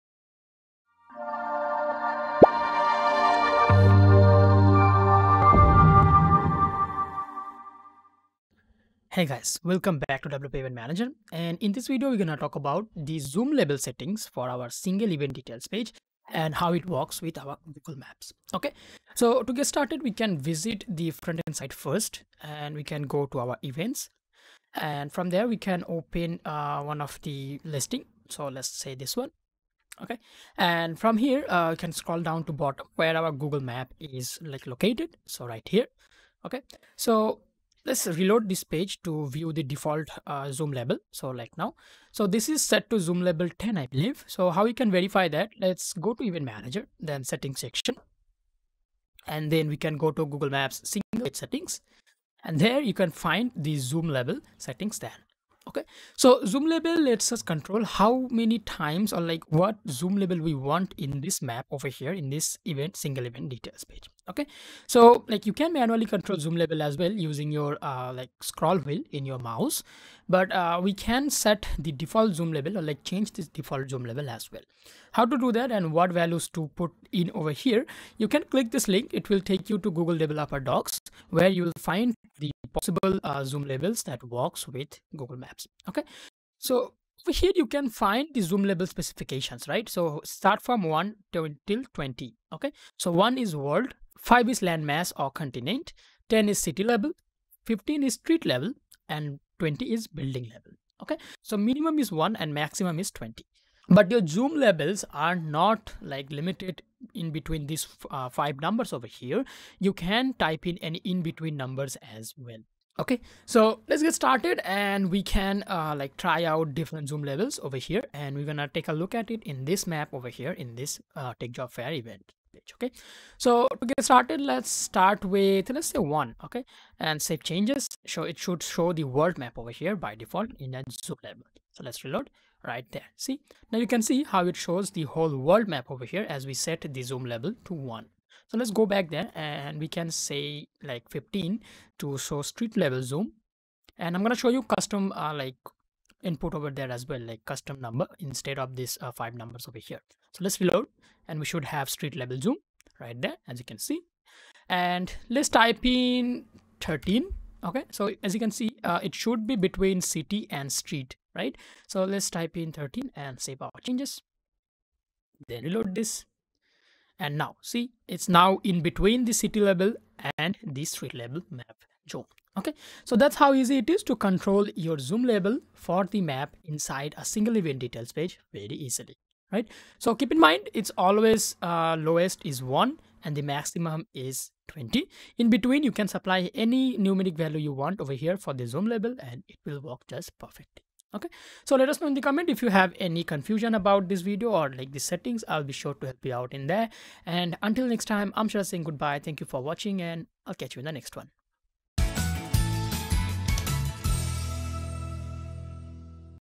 Zoom Level Settings For Single Event Details Page Work With Google Map
The zoom level is something offered by Google maps and with our Google Maps add-on, Different zoom level defines different zoom values which are following.
   1: World
   2: Landmass/continent
   3: City
   4: Streets
    5: Buildings
Starting from 1 to 20 you can choose any number to set the default zoom level for your map at the single event details page